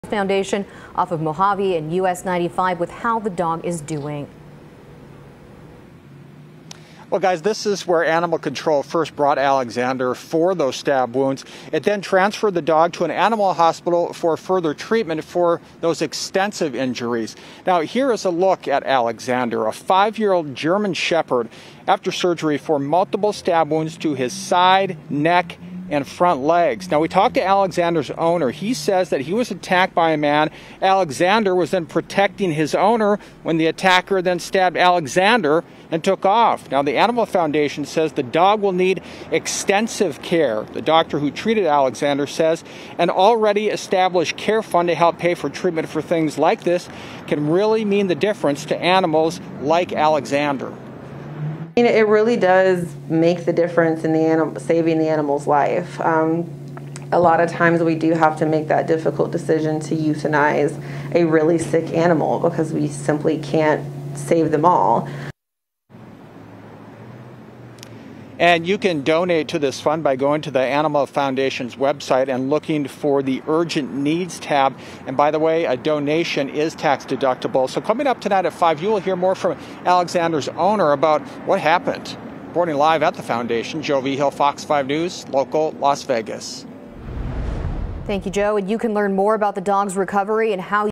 Animal Foundation starts medical fund for German Shepherd with multiple stab wounds
The Animal Foundation has started a medical fund for a German shepherd that sustained multiple stab wounds while trying to protect his owner.